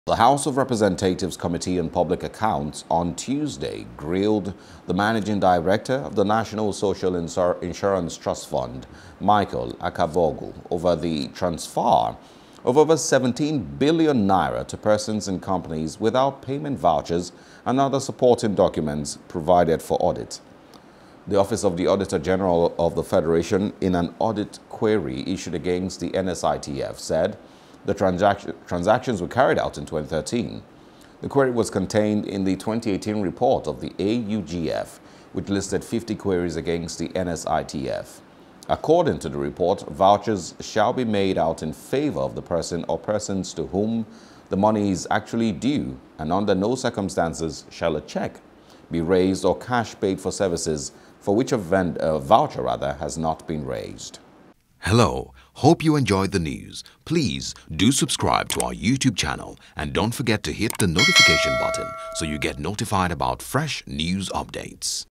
NSITF Faces Reps Over Fresh N17BN Illegal Transfer | BUSINESS
The House of Representatives’ Committee on Public Accounts, on Tuesday, grilled the Managing Director of the National Social Insurance Trust Fund, Michael Akabogu, over the transfer of over N17Bn to persons and companies without payment vouchers and other supporting documents provided for audit.